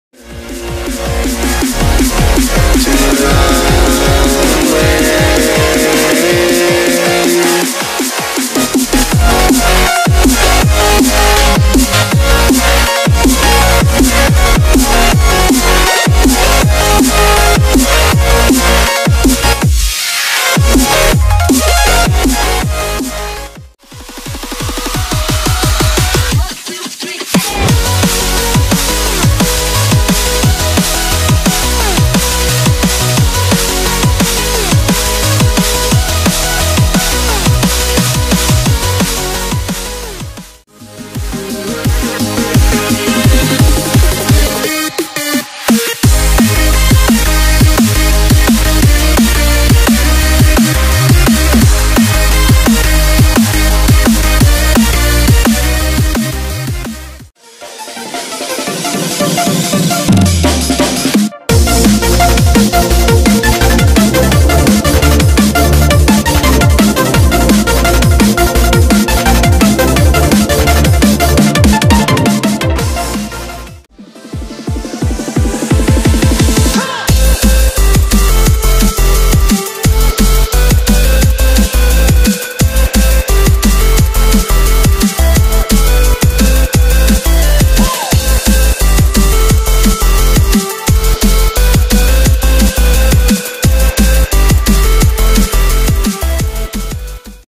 5 NCS Drum and Bass Roblox Music Codes!
As the name suggests none of these are copyrighted and are free to use.

Music listed in the video

SOOBSCROOB